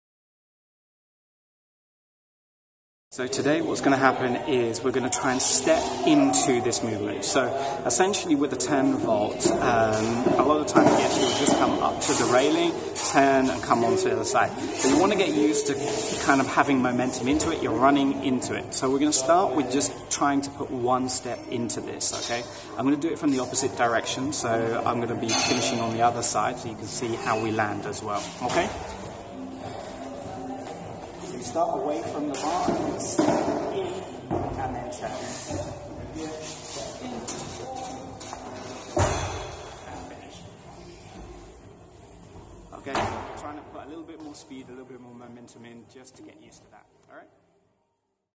How to Turn Vault #20 - Momentum Parkour Tutorial. [140]
🎓 A Tutorial on how to Turn Vault (Beginner) in Parkour - Momentum.
🥷 The Parkour Turn Vault Series. Episode #20

Momentum.

Now we need to get used to having a little momentum included into the turn vault. We can start with an extra step into movement and build up.

Remember, as always to practice both sides and try to use the maximum amount of momentum you can by transitioning straight into the turn vault without any pauses.

　　  ＬＯＮＤＯＮＰＡＲＫＯＵＲ